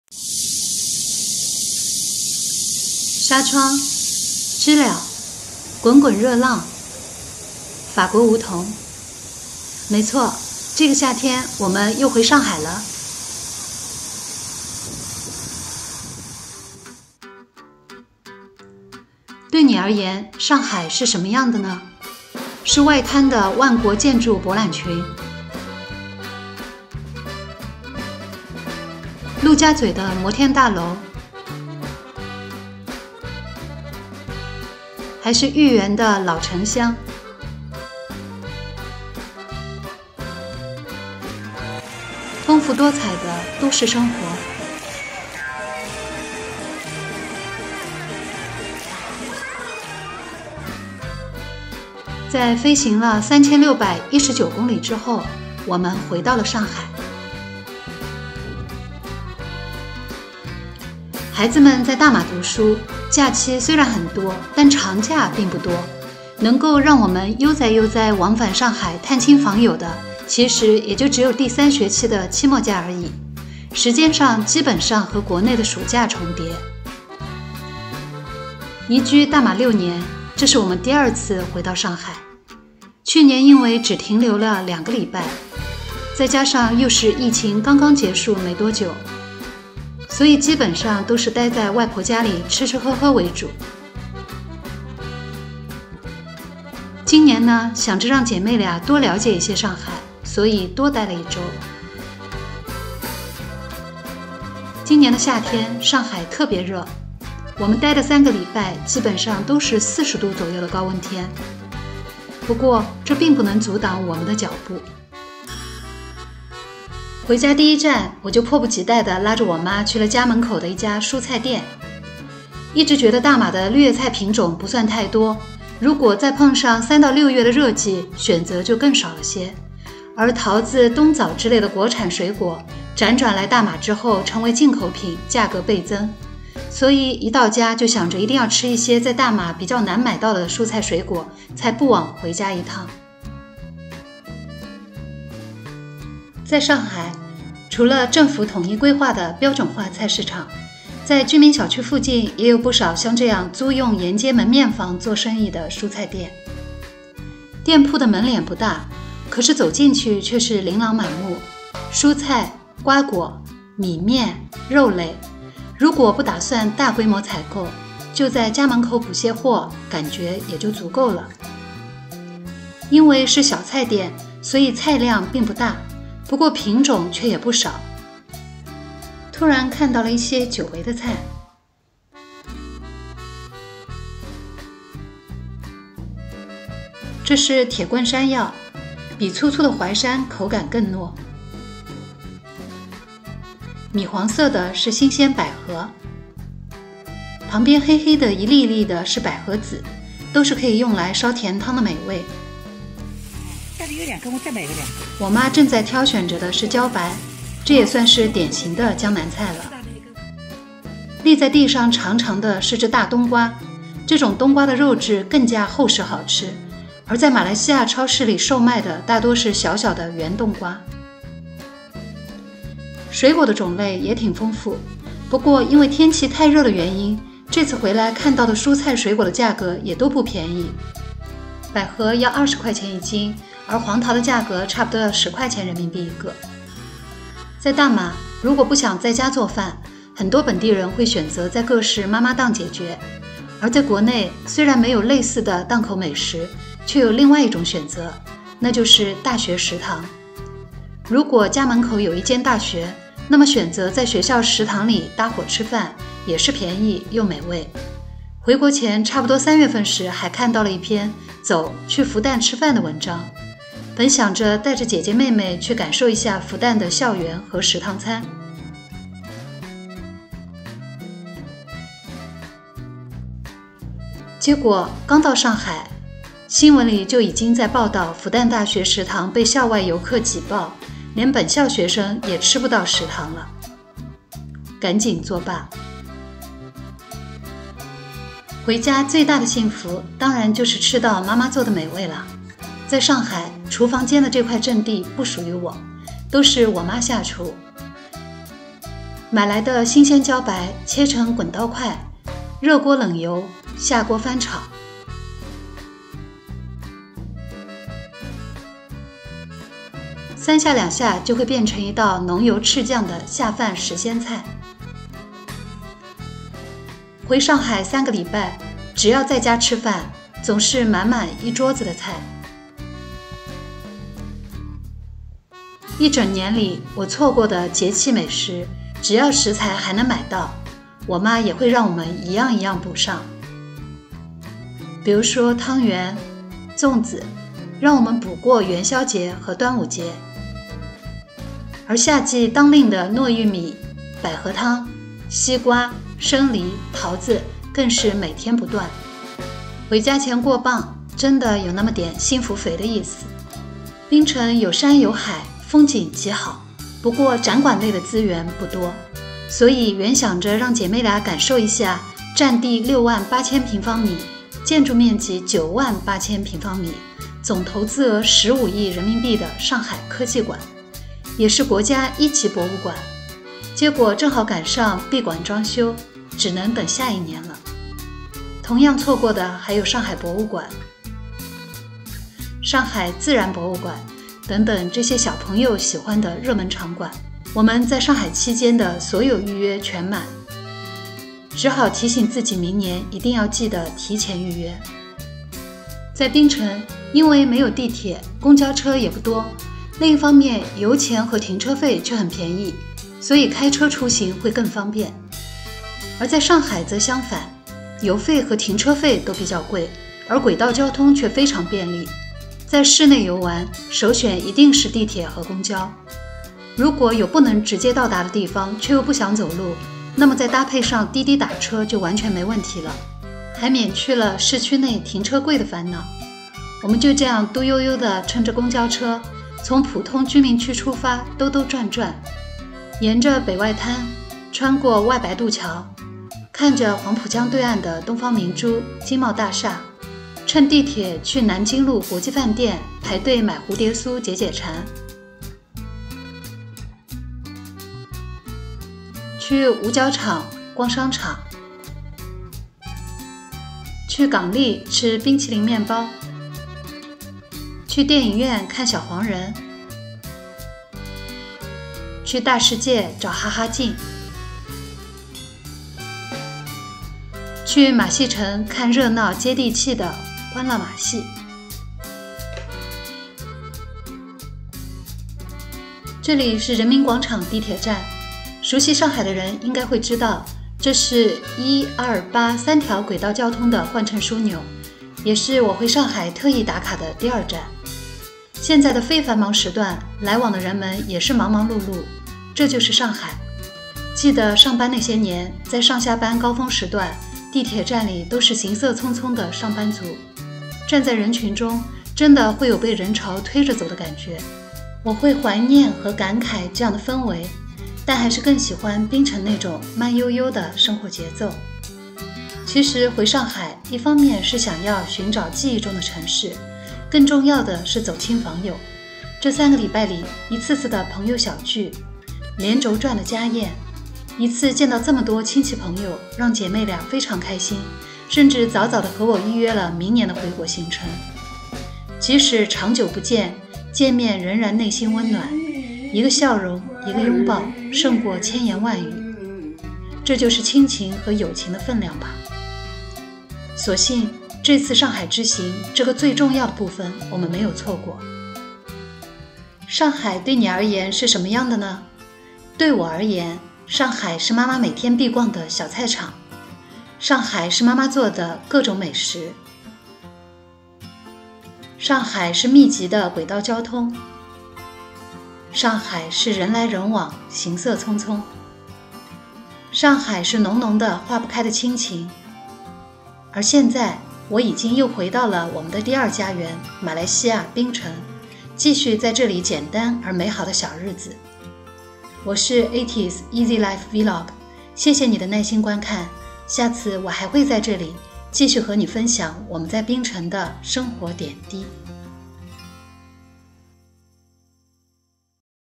48. 第二家园大马 VS 老家上海，我们更想念哪里？malaysia
移居大马槟城已有6年，这次是我们第二次回到上海。上海的繁华与便捷，充满都市节奏的生活，和槟城的慢节奏、自然风光形成了鲜明对比。这段视频记录了我们从大马回到上海的夏日归程，探讨槟城和上海之间的生活差异，以及我们心中对两地的真实感受。

**是上海的高楼林立、热闹的街景让我们怀念，还是槟城的碧海蓝天和悠闲的生活更让人向往？**在这次旅程中，我们对比了两地的日常、饮食、交通、亲情，以及对各自生活方式的喜爱。希望这段分享能够引发你对“第二家园”和“故乡”之间情感的共鸣。